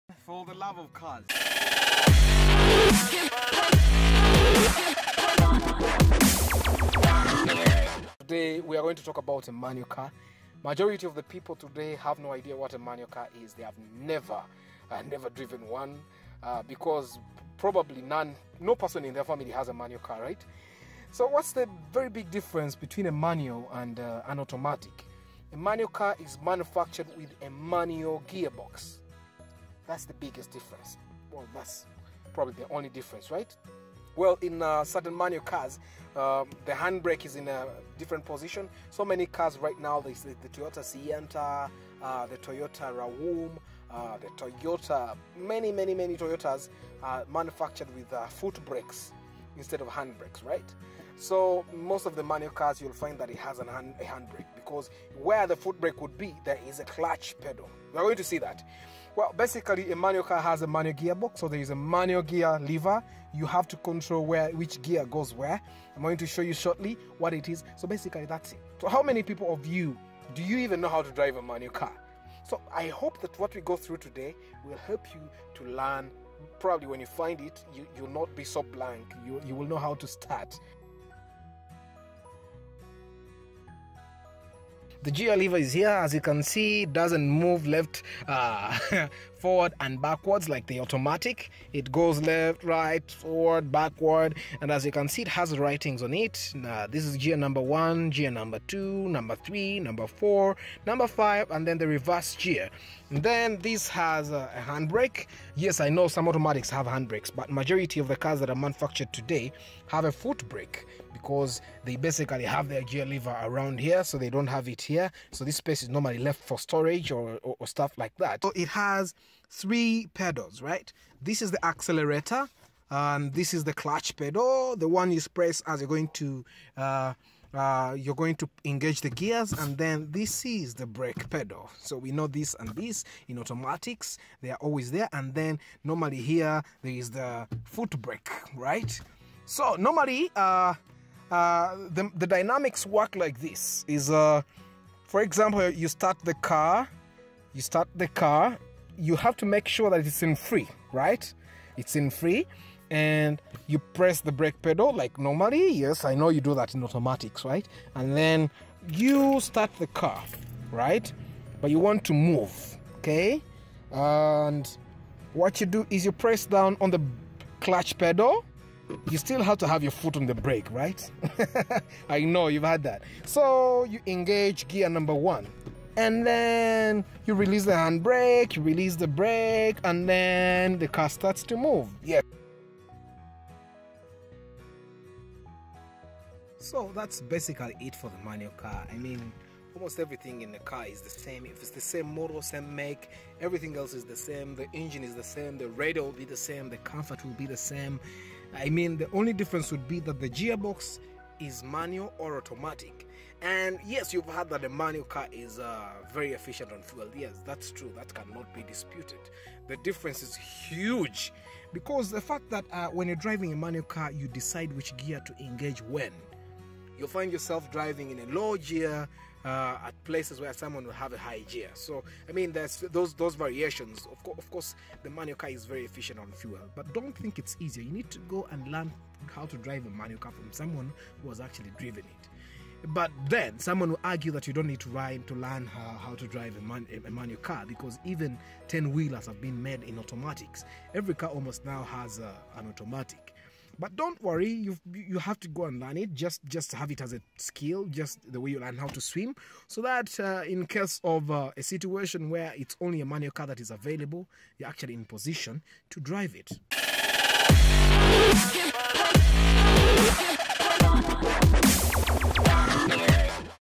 The difference between a Manual and Automatic Car
Showing you The Difference between a Manual and an Automatic Car is what we are all about in this video. You will never be stuck again when there is a manual car around.

Let me know what more you want to see on this channel!